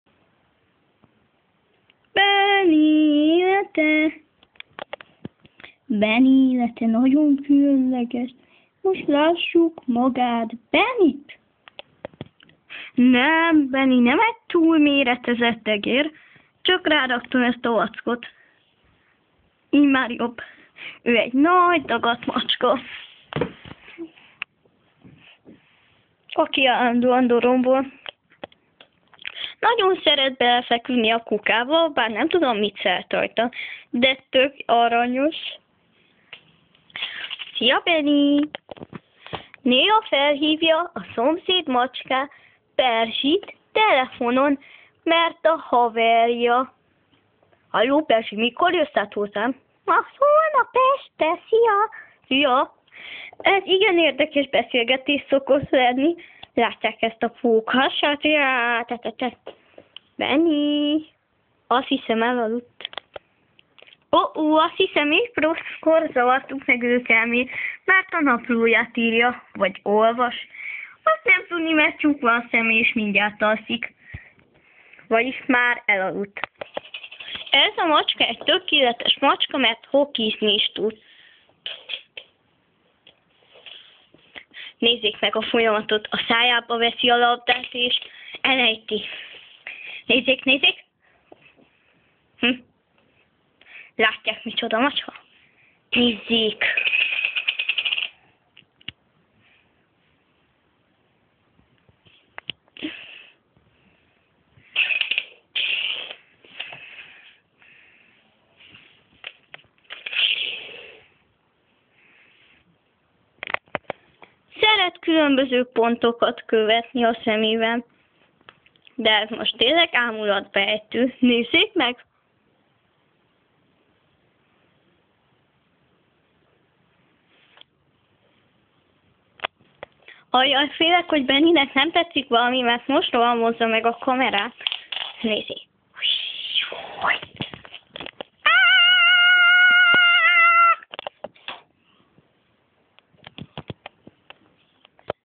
Benny élete
A hugom különös dologra vállakozik, megpróbálkozik macskám bonyolult életének bemutatására :D